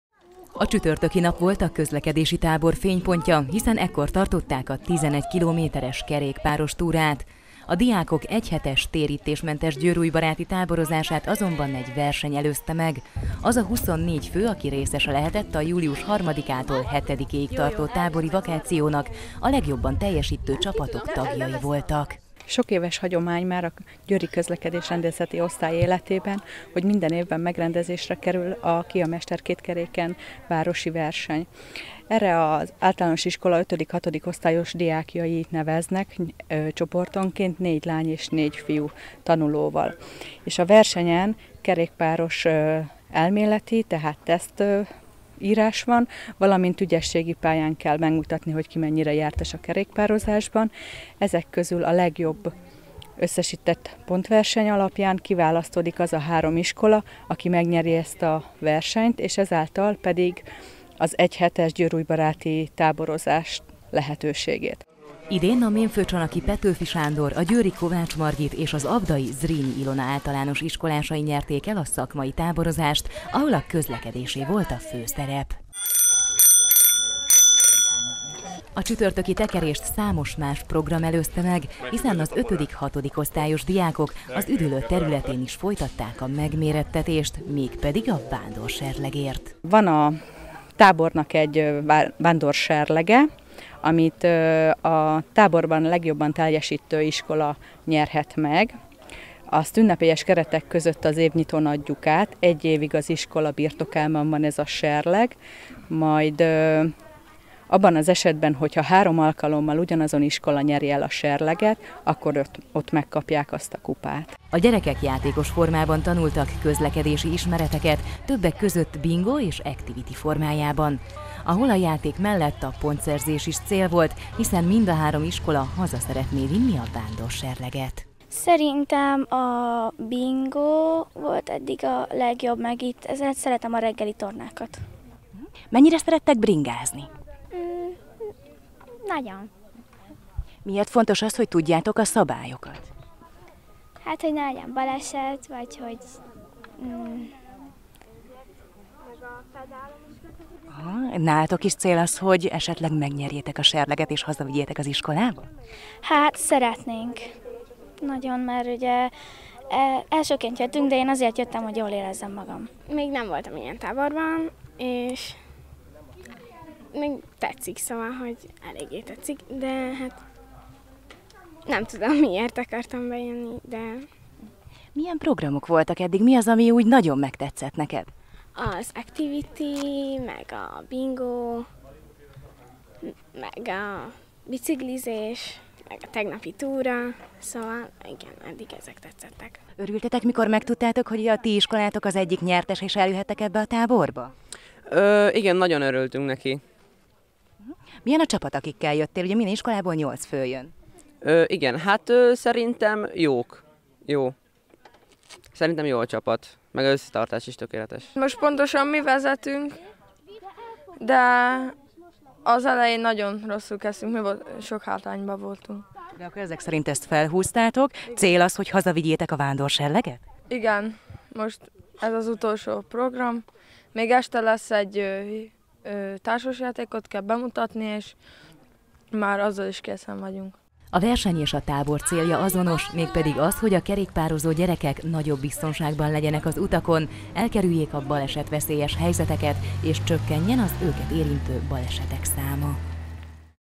Vándorserlegért hajtottak
24 diák vett részt az egy hetes, térítésmentes, közlekedési táborban. A Győri Rendőrkapitányság Közlekedésrendészeti Osztálya által szervezett „Ki a mester két kerekén” című közlekedésbiztonsági vetélkedő legjobban teljesítő 3 csapata lehetett ott a július 3-tól 7-ig tartó, győrújbaráti táborban. A nyertesek a múlt héten, olyan programokon vehettek részt, melyek a rendőrség munkáját mutatták be és mivel szakmai táborról volt szó, a közlekedés volt a középpontban.